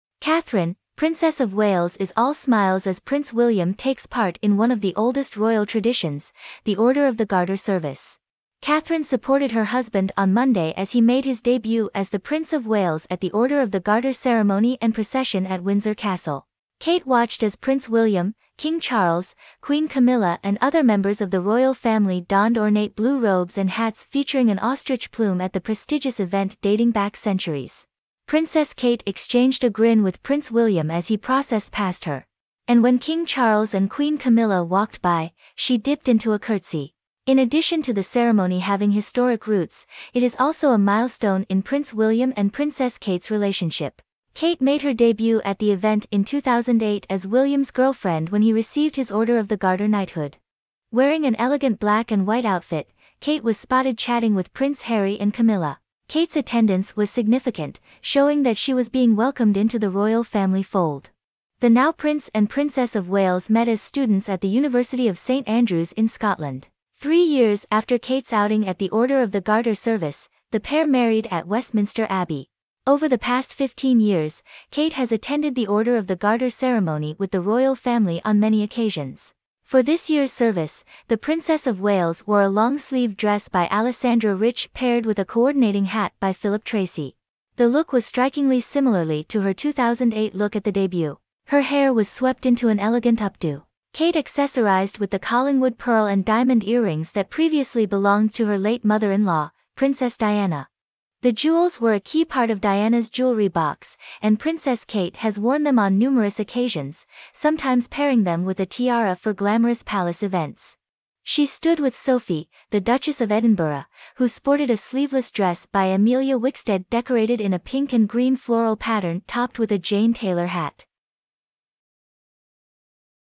Princess Catherine Returns to Royal Event Where She's Supported Prince William Since Before Marriage
Catherine, Princess of Wales is all smiles as Prince William takes part in one of the oldest royal traditions - the Order of the Garter service.

Catherine supported her husband on Monday as he made his debut as the Prince of Wales at the Order of the Garter ceremony and procession at Windsor Castle. Kate watched as Prince William, King Charles, Queen Camilla and other members of the royal family donned ornate blue robes and hats featuring an ostrich plume at the prestigious event dating back centuries.

Princess Kate exchanged a grin with Prince William as he processed past her. And when King Charles and Queen Camilla walked by, she dipped into a curtsy.

In addition to the ceremony having historic roots, it is also a milestone in Prince William and Princess Kate's relationship. Kate made her debut at the event in 2008 as William's girlfriend when he received his Order of the Garter knighthood. Wearing an elegant black and white outfit, Kate was spotted chatting with Prince Harry and Camilla.

Kate's attendance was significant, showing that she was being welcomed into the royal family fold.

The now-Prince and Princess of Wales met as students at the University of St. Andrews in Scotland. Three years after Kate's outing at the Order of the Garter service, the pair married at Westminster Abbey.

Over the past 15 years, Kate has attended the Order of the Garter ceremony with the royal family on many occasions.

For this year's service, the Princess of Wales wore a long-sleeved dress by Alessandra Rich paired with a coordinating hat by Philip Treacy. The look was strikingly similarly to her 2008 look at the debut. Her hair was swept into an elegant updo.

Kate accessorized with the Collingwood pearl and diamond earrings that previously belonged to her late mother-in-law, Princess Diana. The jewels were a key part of Diana's jewelry box, and Princess Kate has worn them on numerous occasions — sometimes pairing them with a tiara for glamorous palace events.

She stood with Sophie, the Duchess of Edinburgh, who sported a sleeveless dress by Emilia Wickstead decorated in a pink and green floral pattern topped with a  Jane Taylor hat.